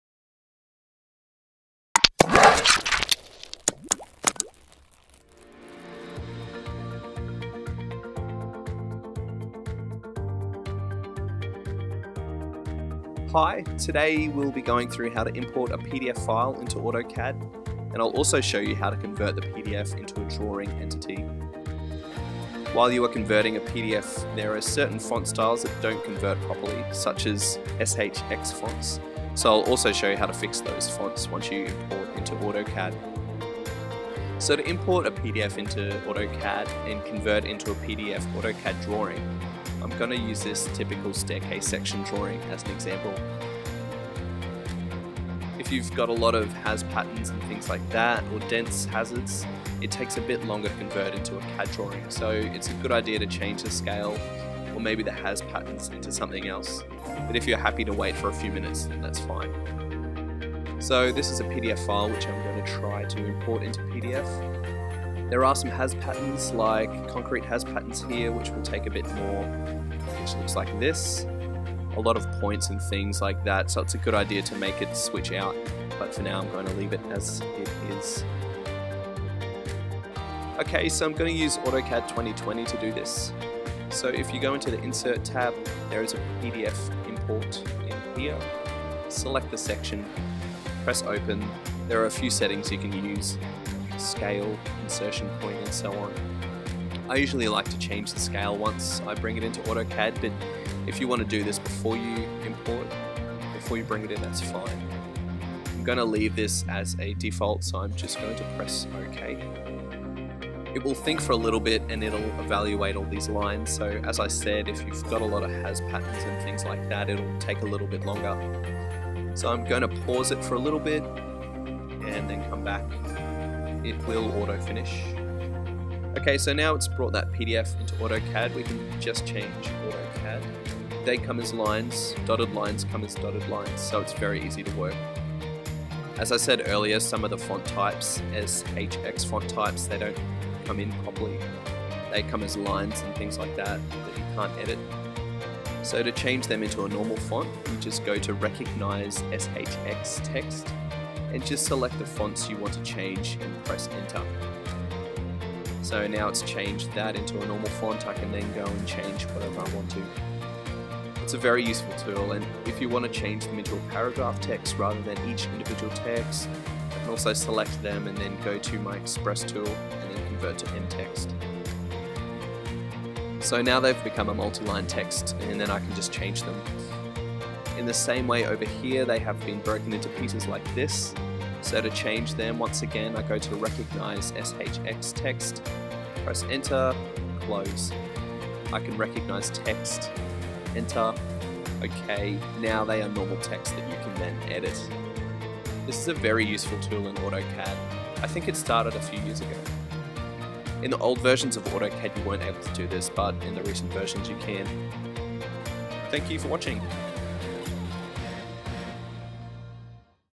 Autodesk - How to convert PDF to AutoCAD SHX to MTXT
Learn how to import a PDF file into AutoCAD and how to convert the PDF into a drawing.